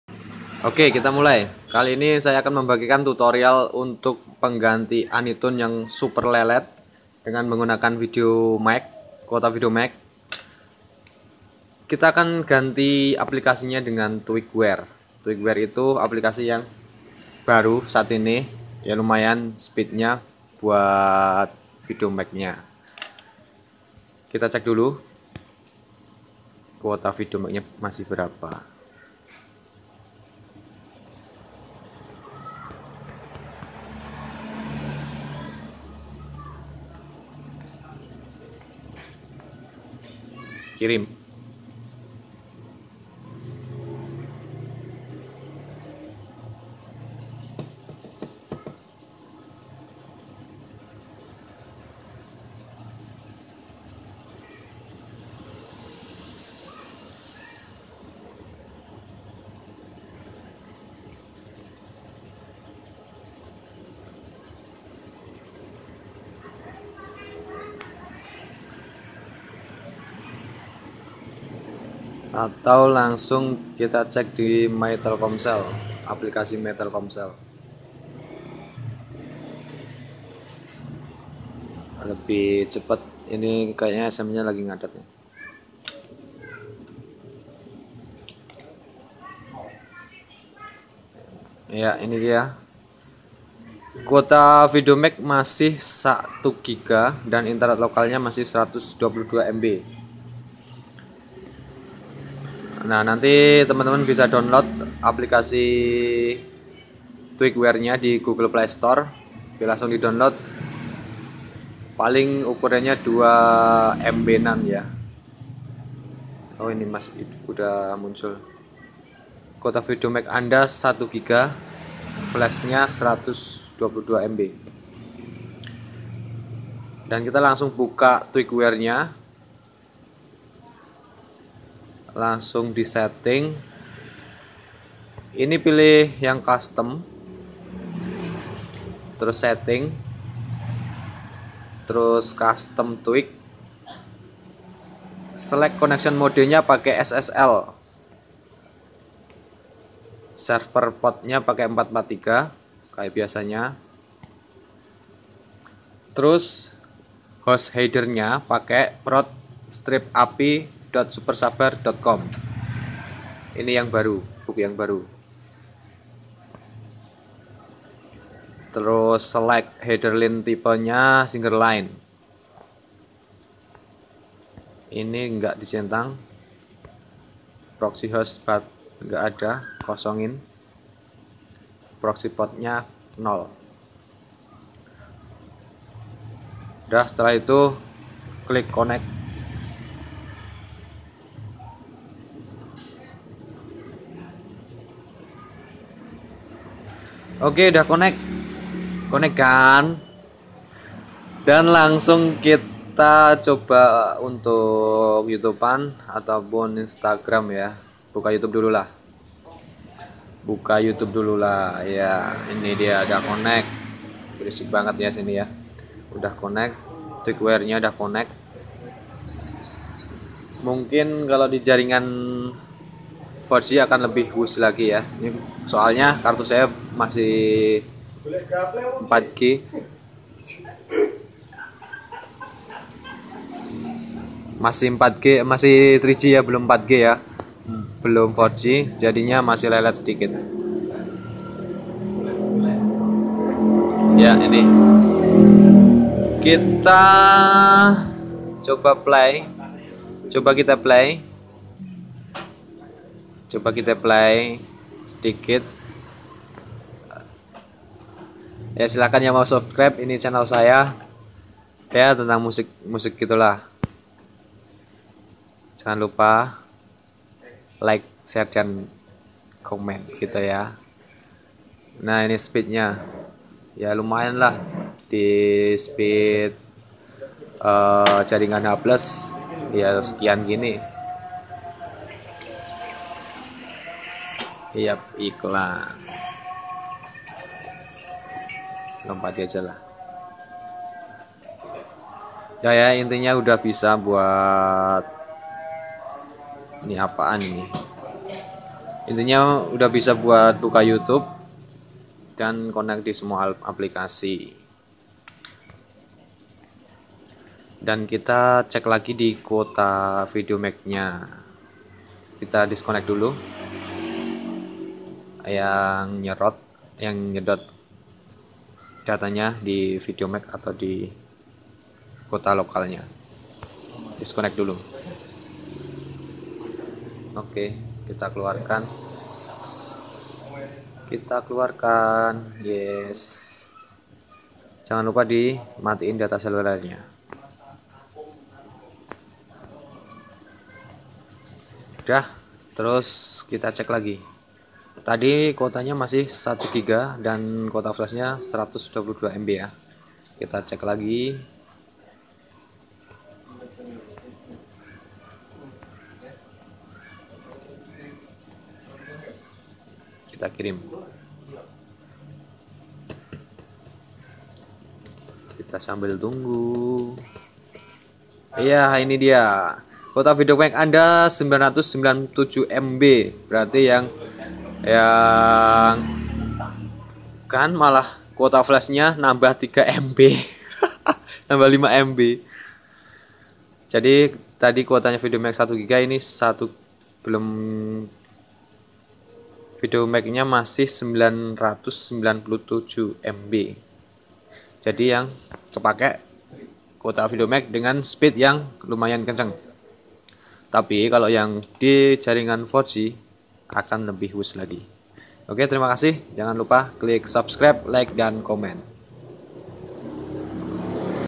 CARA BARU TELKOMSEL VIDEOMAX SPEED WUZZ DENGAN APLIKASI TWEAKWARE PENGGANTI ANONYTUN
Cara baru speed wuz telkomsel videomax. dengan menggunakan aplikasi pengganti anonytun yaitu TWEAKWARE, bisa di download di playstore

ASKfm @shokiebgankz